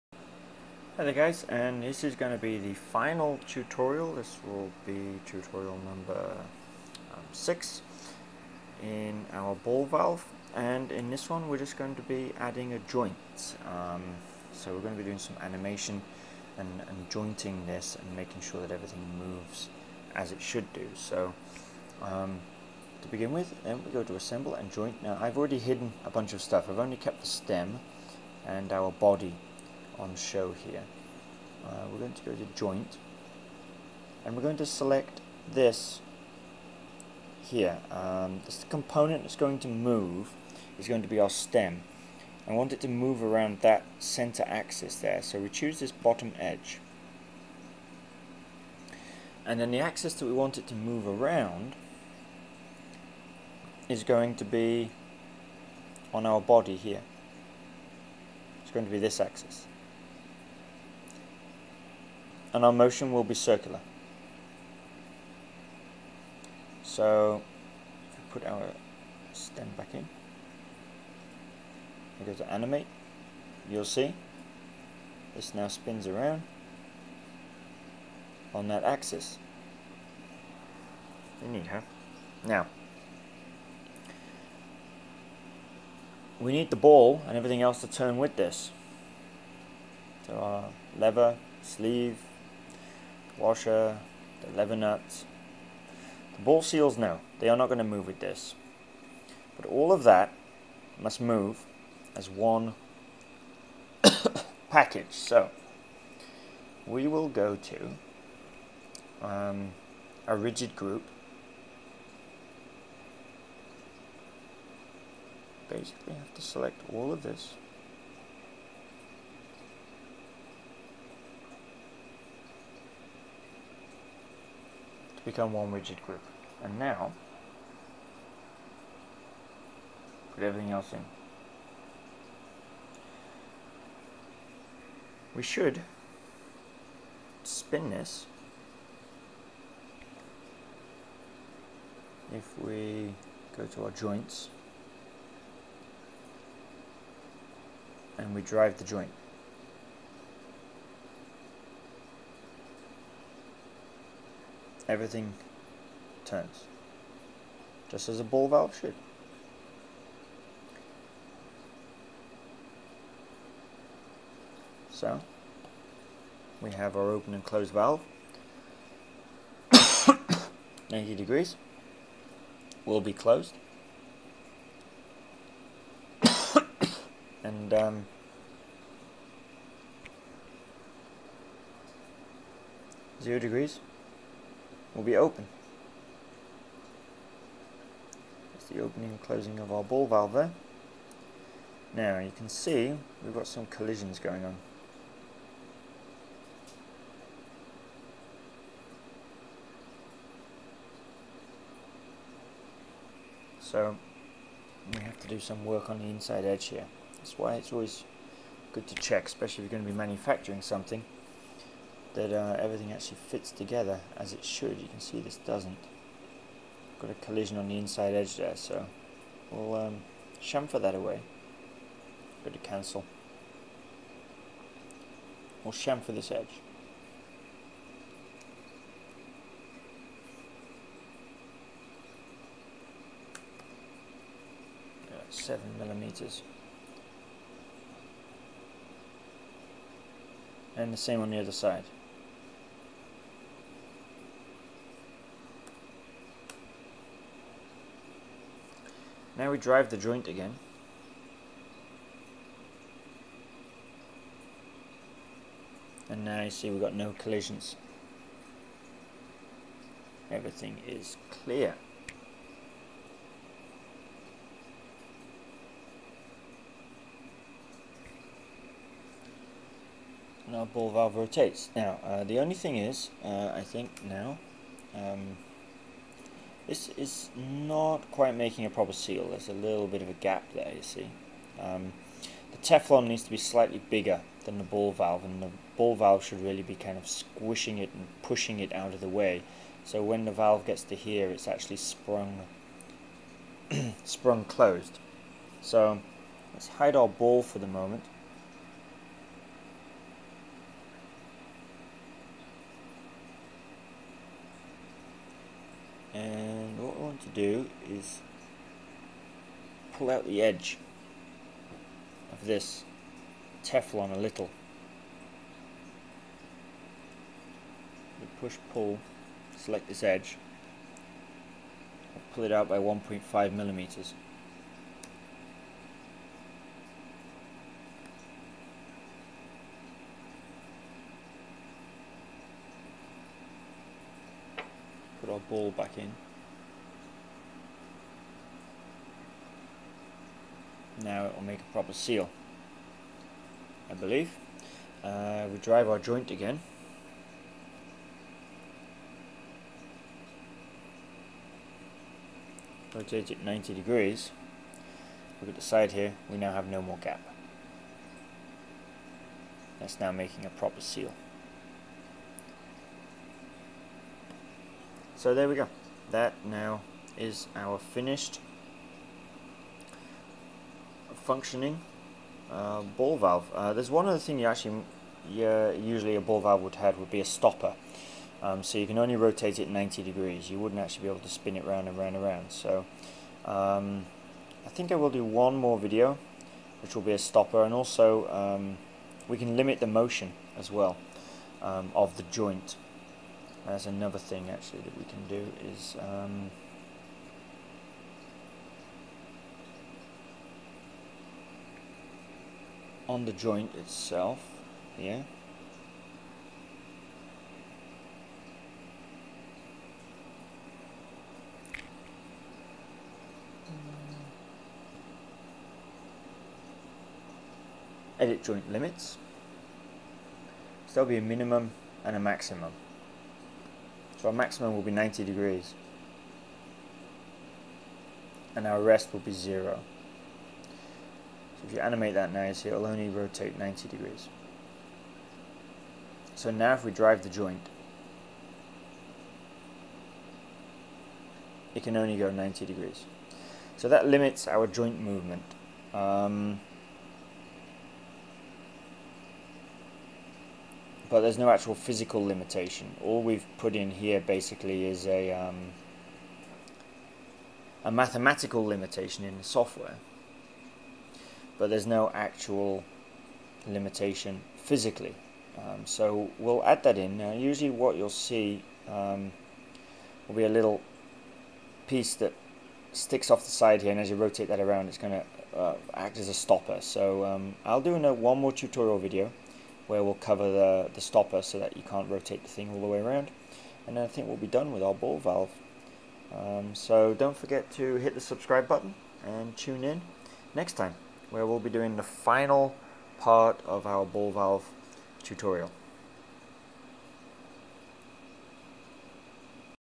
Fusion 360 - Ball Valve - Tutorial 06
Fusion 360 - Jointing the ball valve and checking for collision. Final adjustments.